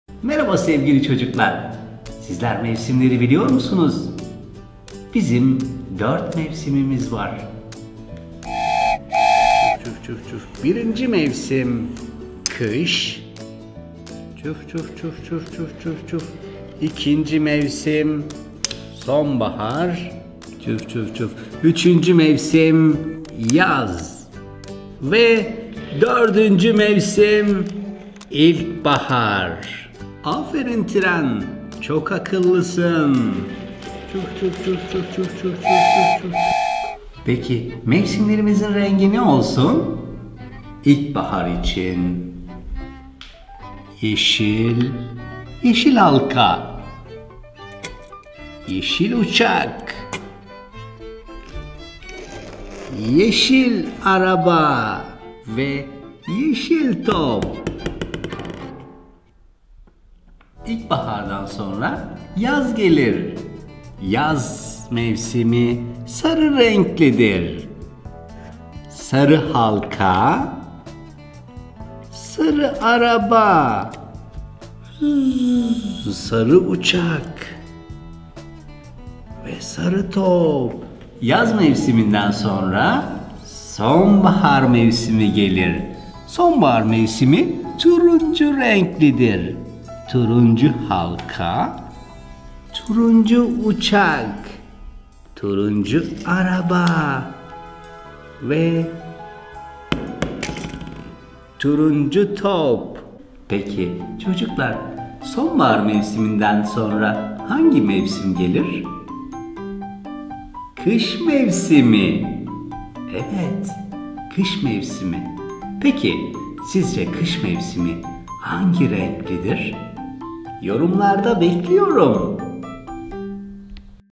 Harika kanatlar. Mevsimleri, renkleri ve sayıları öğreniyoruz
Harika kanatlar ile eğitici video izleyelim. Çocuklar için yeni eğitici video kanalımızda. Nail Baba'yla mevsimleri öğrenelim. Tren oyuncak bizlere yardım edecek. Sizler mevsimleri biliyor musunuz? Dört mevsimimiz var; birinci mevsim kış, ikinci mevsim sonbahar, üçüncü mevsim yaz ve dördüncü mevsim ilkbahar. Aynı zamanda Nail Baba bizlere renkleri ve sağıları öğretiyor. Çocuklar sizin en sevdiğiniz mevsim hangisidir? Yorumlarınızı bekliyoruz.
Nail Baba ile çocuk oyunlarını izle